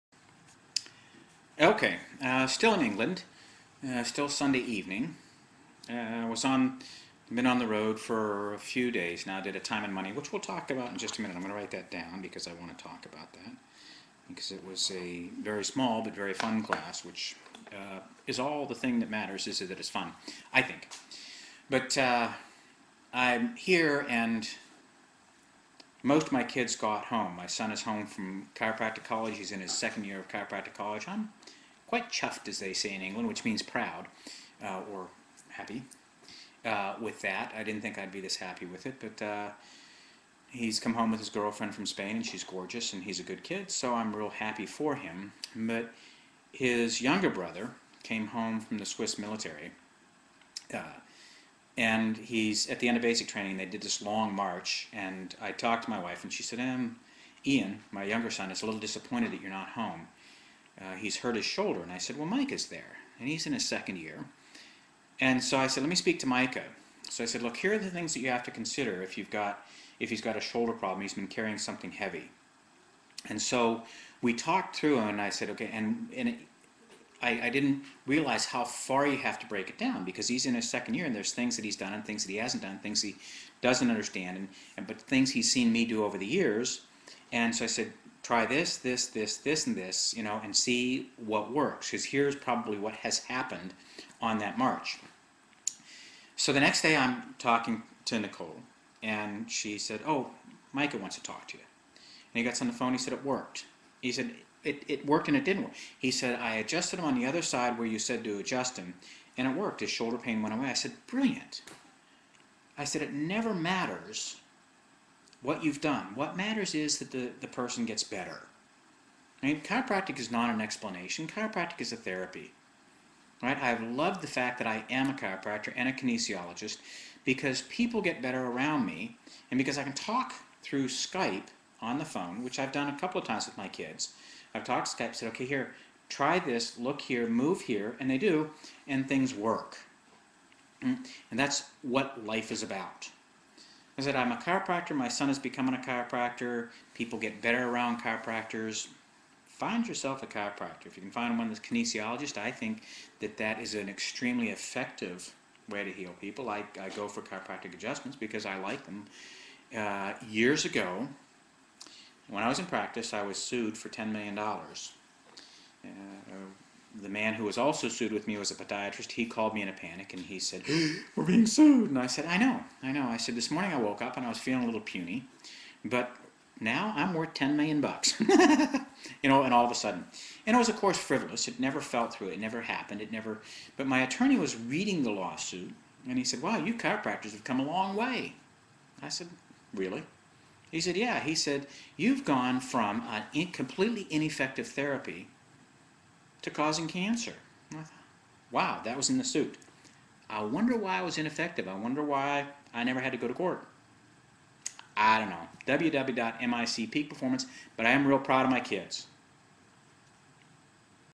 Being Proud of your Kids • Chiropractic Teaching • Mick MacKenzie • Self Actualization
Chiropractic is not an 'explanation', it's a therapy...Making things work is what life is about... To find out more about Mick's Self-Actualisation Learning ...